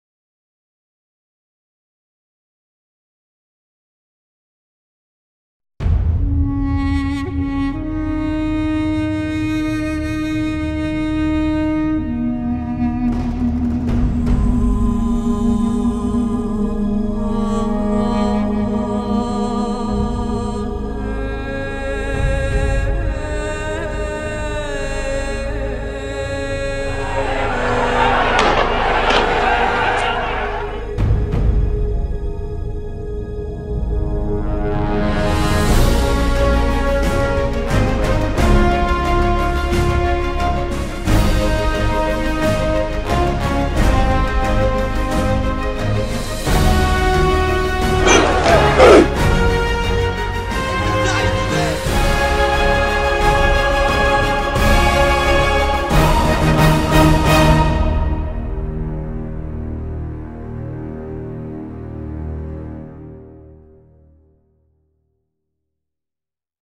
Egyptian Revolution 2011-Trailer
This is to thank all Egyptian who shared and who couldn't in this Revolution, hoping for a better future for our new modern Liberally Egypt.
Created by Amir Fouad